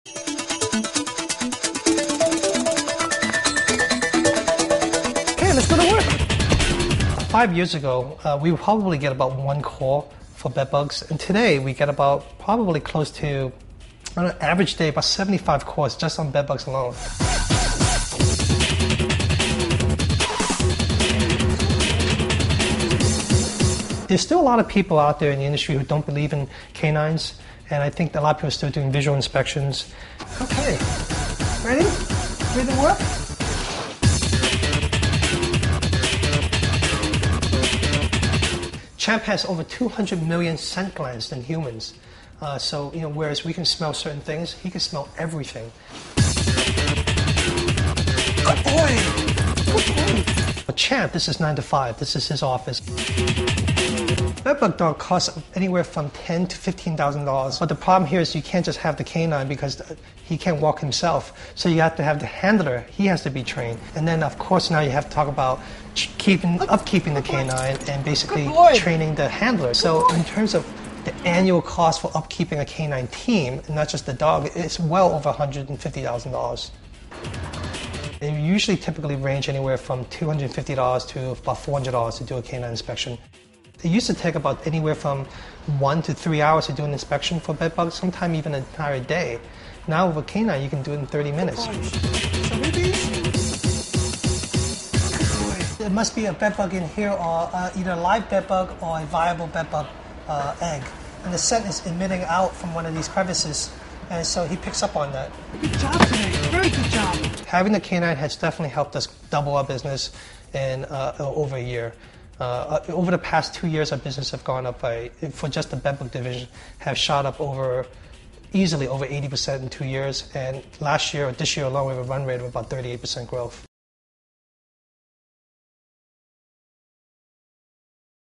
Bedbug dog sniffs up profits
This bedbug canine is making one pest control company tons of dollars by hunting down pesky critters.